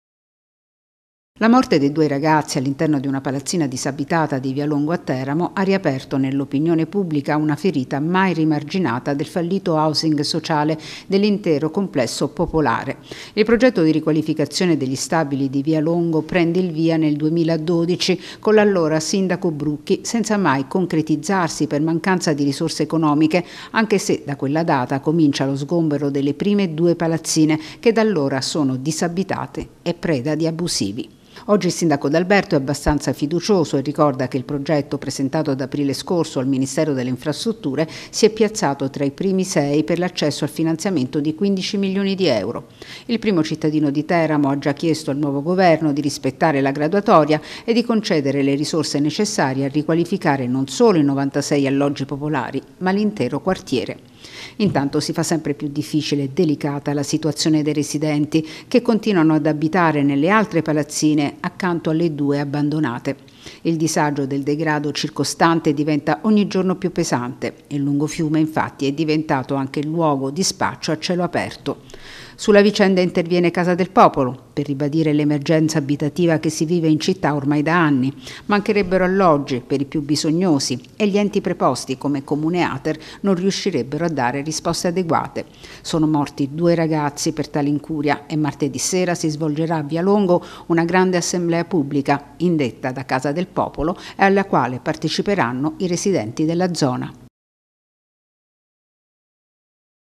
Tragedia di via Longo: protestano residenti e forze politiche
Domani martedi  6 dicembre alle ore 20, in via Longo a Teramo, scenderanno in strada residenti e forze politiche per protestare contro il degrado del quartiere e l'emergenza abitativa. La morte di due ragazzi all'interno di una palazzina disabitata nel complesso popolare alle porte della città ha fatto esplodere la rabbia dei cittadini che chiedono l'attuazione del progetto di housing sociale, fermo da tanti anni.